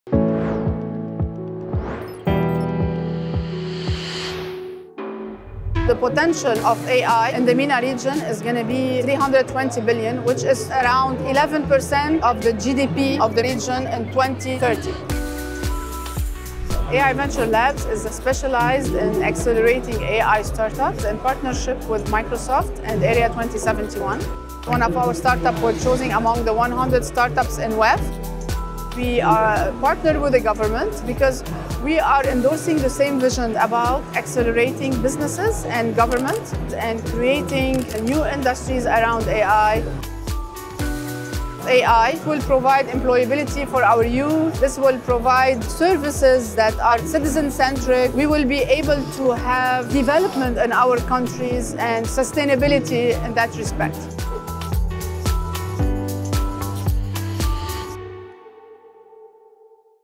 Middle East & Africa A.I. spend of US$ 374 million predicted in 2020
International Data Company predicts Middle East & Africa A.I. spend of US$ 374 million in 2020 with an increase at a compound annual growth rate of 19% by 2023. “The potential of A.I. in the MENA region is going to be US$ 320 billion which is around 11% of the GDP of the region in 2030”, says CEO, Roula Moussa.